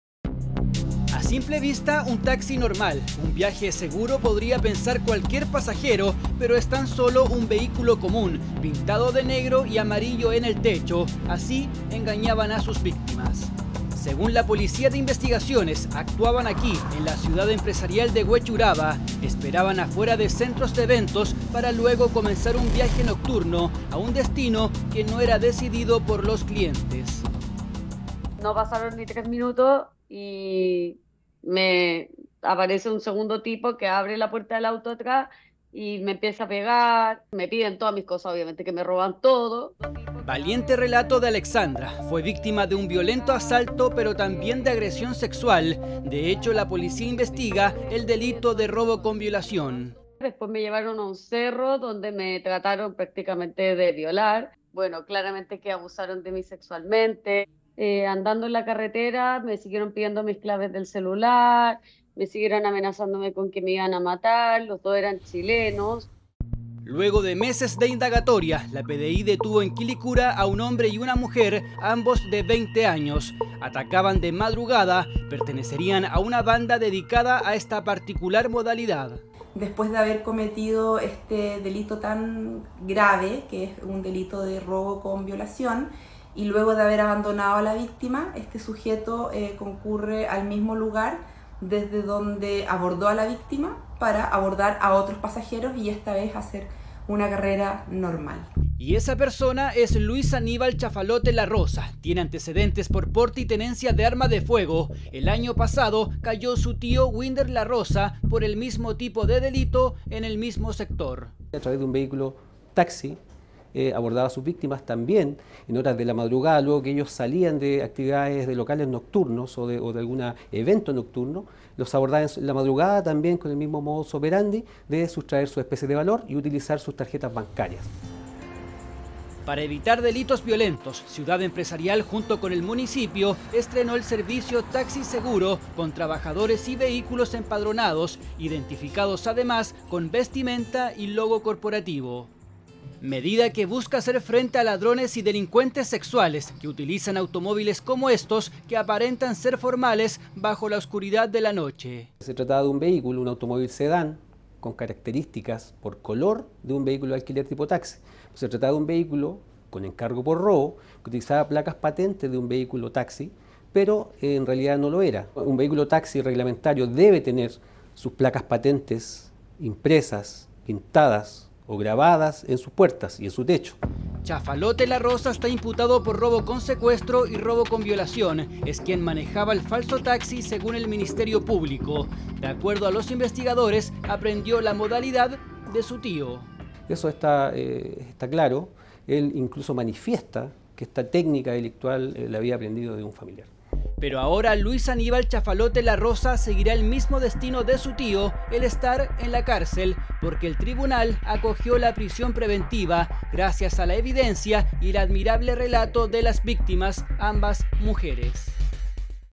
Falso taxista secuestraba y robaba a mujeres
📌Pintaron un vehículo robado de negro y amarillo, para simular un taxi. Así tomaban pasajeros, los secuestraban y robaban sus tarjetas bancarias.

📌Uno de los detenidos es sobrino de un delincuente que ya fue capturado por el mismo delito bajo esta misma modalidad.